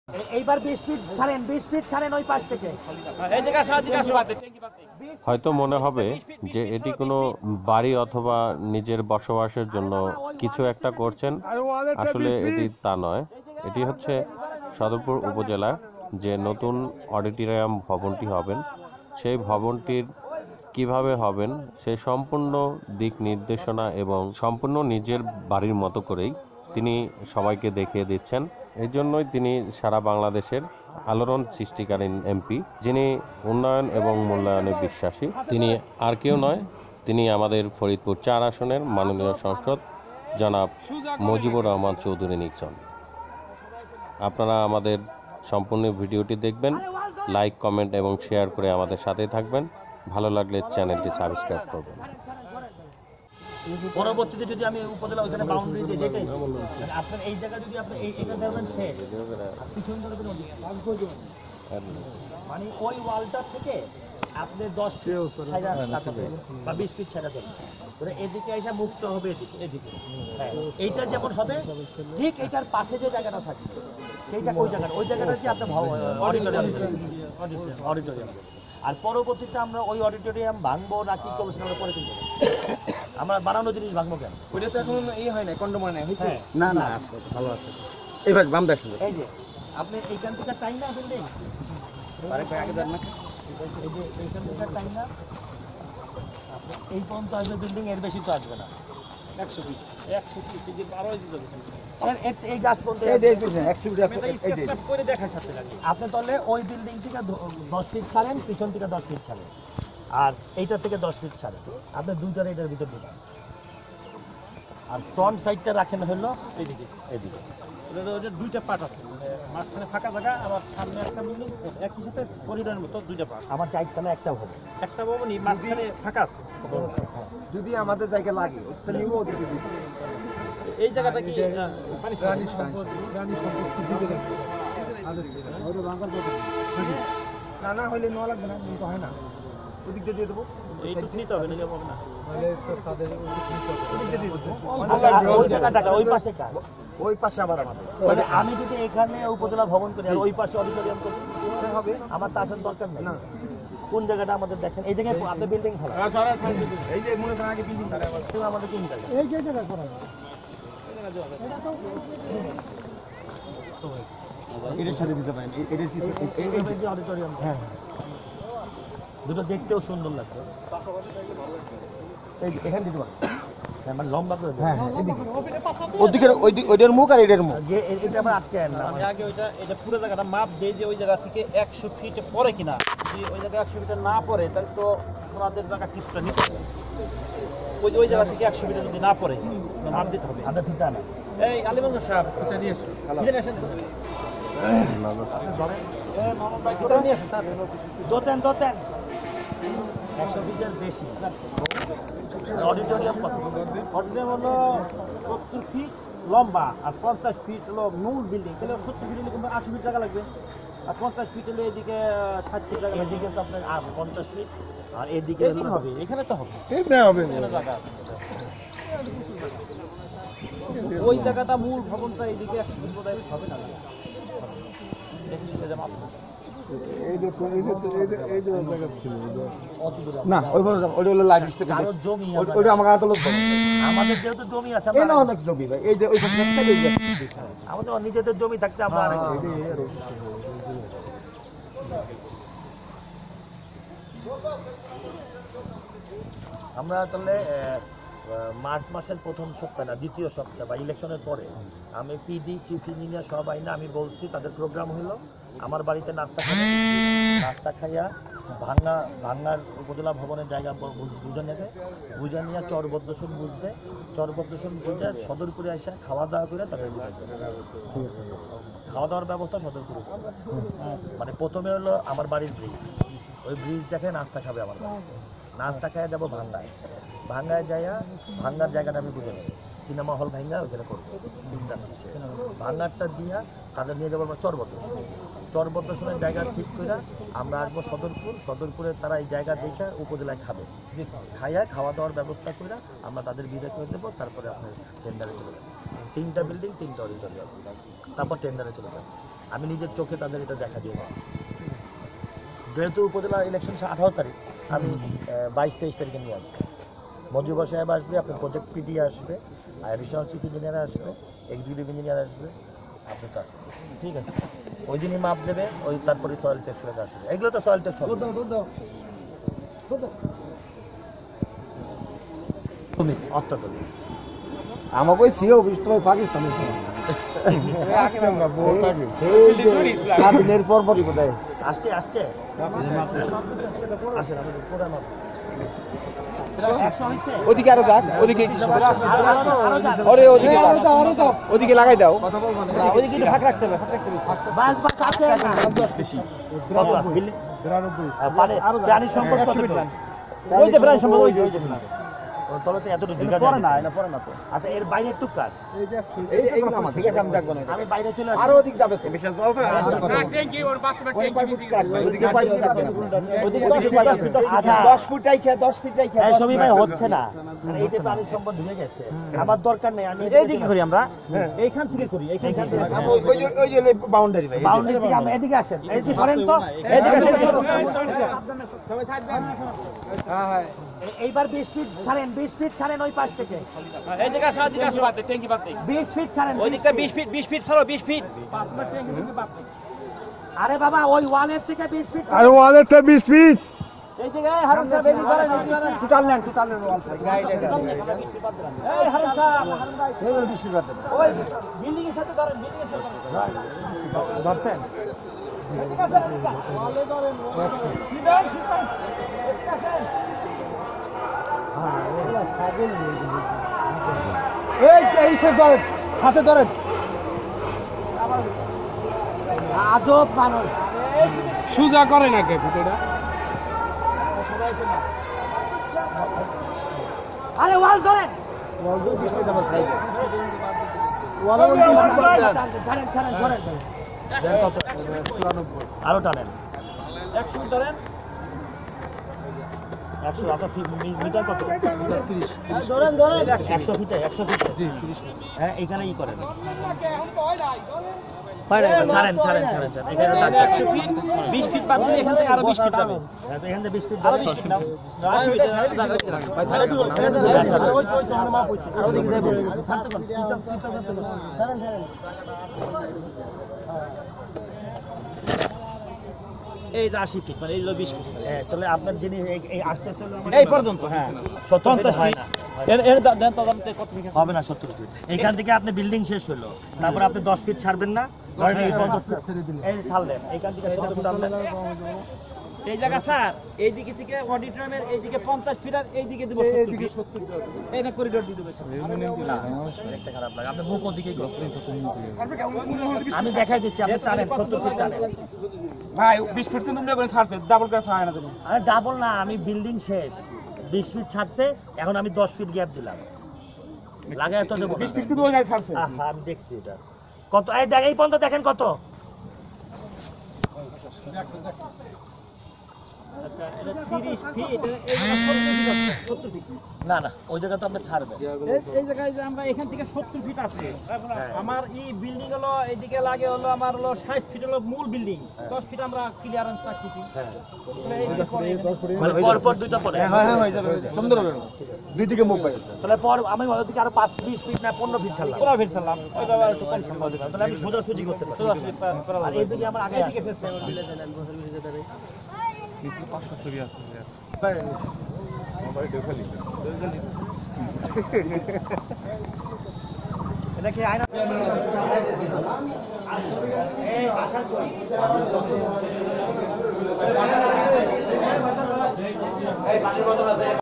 Mp nixon Chowdhury, তিনি যেমন একজন দক্ষ এমপি তেমন দক্ষ ইঞ্জিনিয়ার, উন্নয়ন মূল্যক কাযেও
Mp nixon Chowdhury, তিনি যেমন একজন দক্ষ এমপি তেমন দক্ষ ইঞ্জিনিয়ার, উন্নয়ন মূল্যক কাযেও, নিজেই দেখিয়ে দিচ্ছেন কি ভাবে হবে সদরপুর উপজেলা অডিটোরিয়াম ভবন, নির্মাণ  সবাই লাইক শেয়ার করবেন এবং চ্যানেলটি সাবস্ক্রাইব করবেন ধন্যবাদ